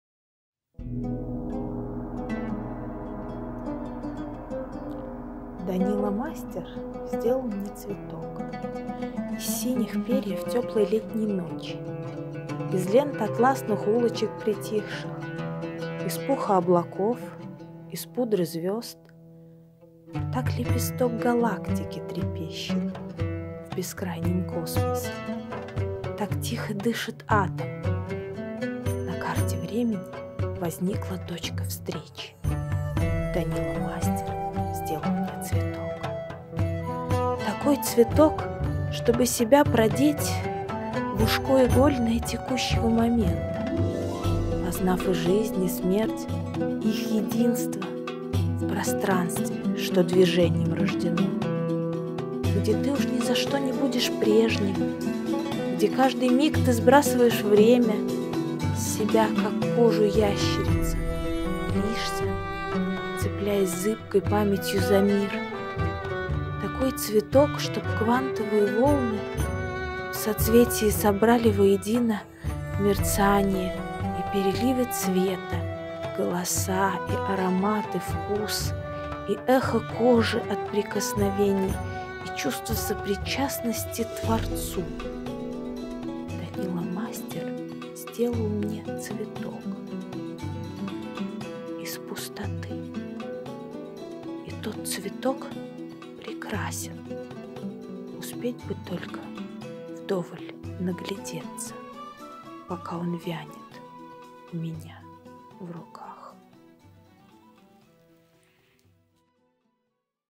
Данила - мастер [Анастасия Баталова]
Это визуализация одного из стихов, прозвучавших на вечере.